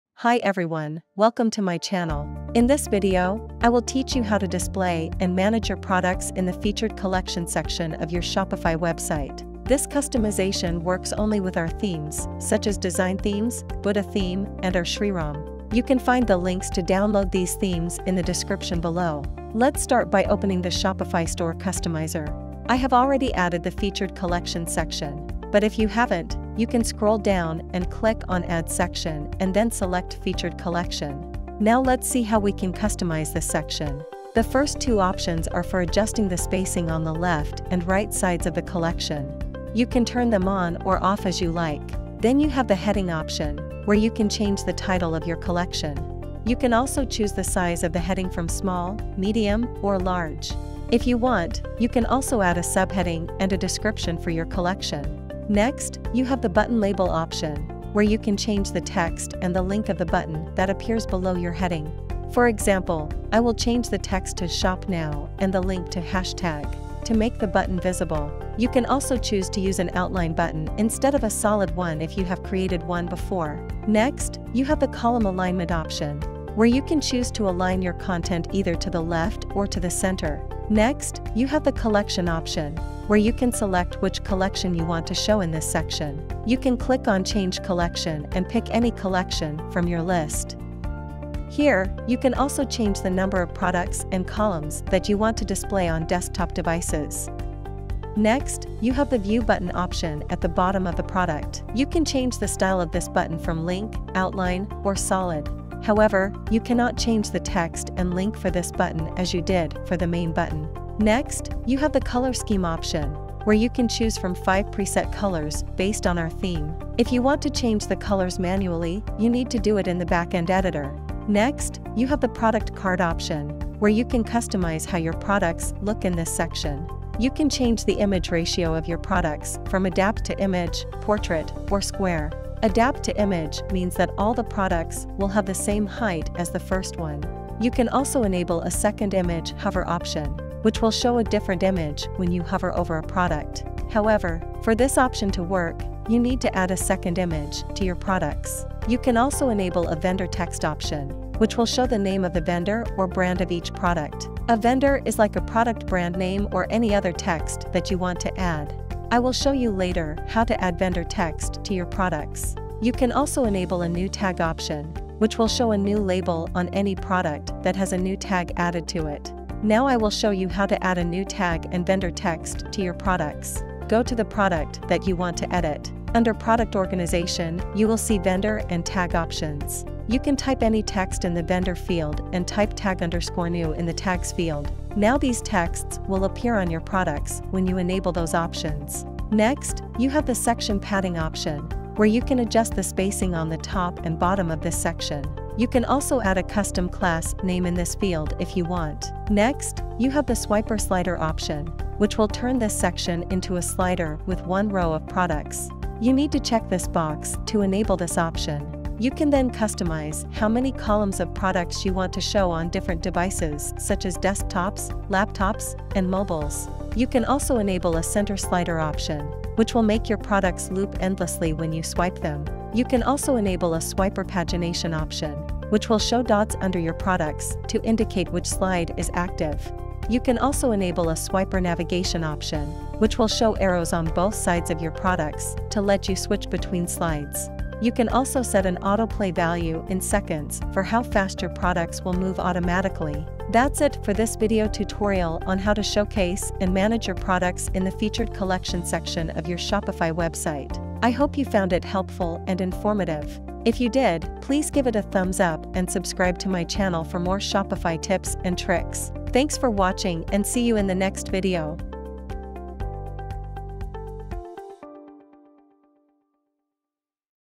How to Customize Your Featured Collection Section on Shopify 2.0
Watch this video to learn How to Customize Your Featured Collection Section on Shopify 2.0